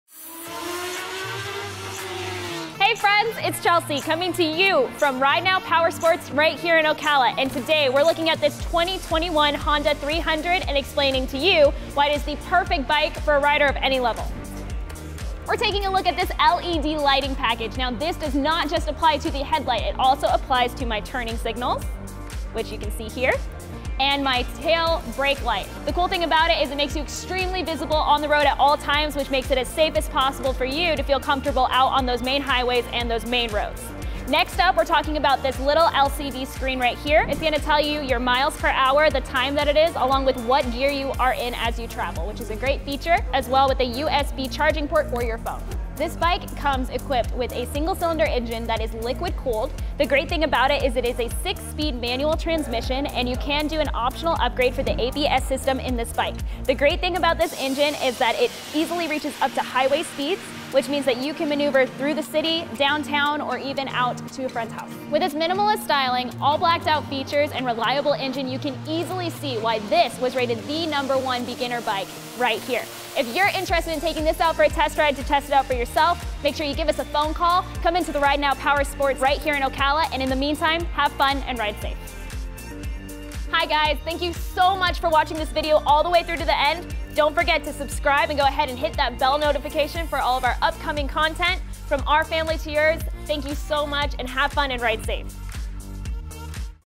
This BEGINNER BIKE is perfect for any rider!
Check out the Honda Rebel 300! We love this lightweight bike for it's all blacked out features, LCD Screen, and LED Lighting Package!

Check out our video to learn more, and don't forget to give RideNow Powersports of Ocala a call at: